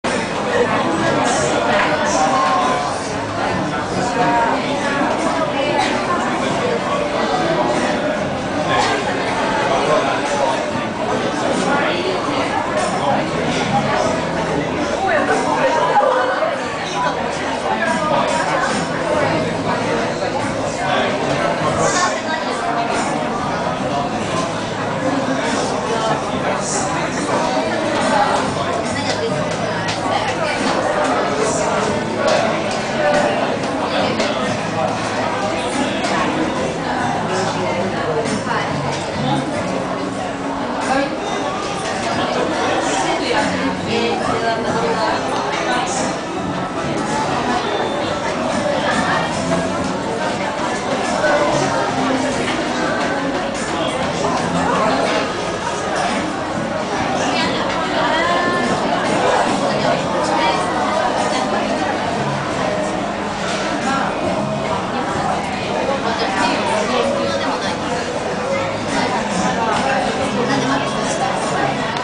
Shibuya Intersection, Tokyo
people crossing in front of Shibuya Station in Tokyo, Japan